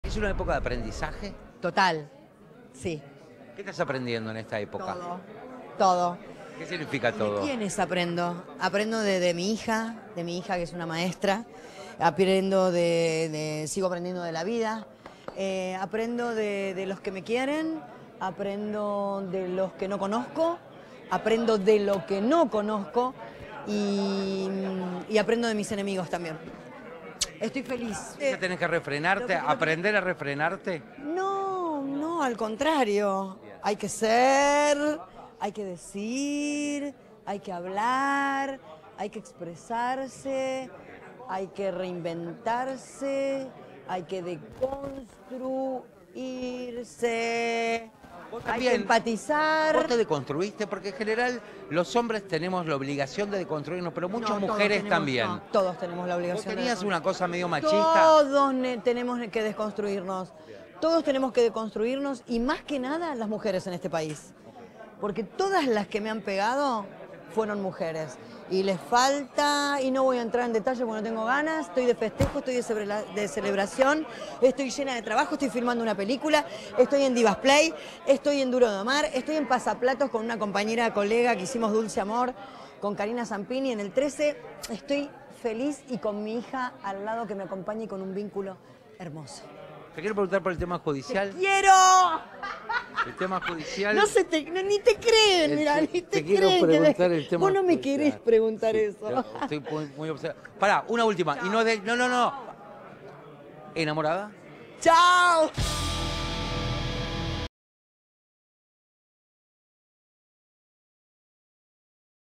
🔥Fernanda Callejón y sus nuevas vibras🧑🏾‍❤️‍💋‍👩
Fernanda Callejón se tomó el tiempo para hablar sobre su nuevo estilo de vida y de cómo se siente hoy en dia.